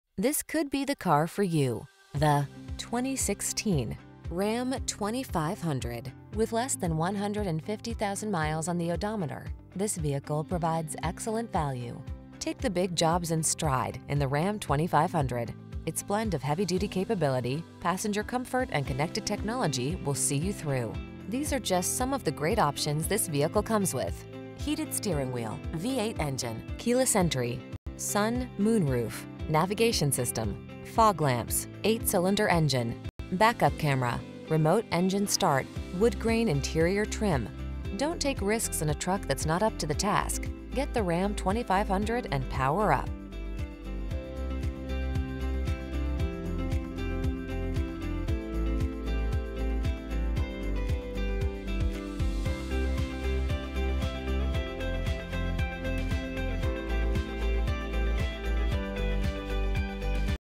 2016 Ram 2500 Laramie Longhorn Atlanta, Marietta, Roswell, Kennesaw, Woodstock GA
Pre-Owned 2016 Ram 2500 Laramie Longhorn available at Ed Voyles CDJR located in 789 Cobb Pkwy SE, Marietta, GA, 30060.
Stock#: 5P52129A - VIN#: 3C6UR5GJ5GG234781

Please mention the stock# 5P52129A when you contact the dealership for this vehicle.

This could be the car for you, the  2016  Ram 2500.  Take the big jobs in stride in the Ram 2500. Its blend of heavy-duty capability, passenger comfort, and connected technology will see you through. These are just some of the great options this vehicle comes with: Heated Steering Wheel
, V8 Engine, Keyless Entry, Sun/Moonroof, Navigation System, Fog Lamps, 8 Cylinder Engine, Back-Up Camera, Remote Engine Start, Woodgrain Interior Trim. Don't take risks in a truck that's not up to the task. Get the Ram 2500 and power up.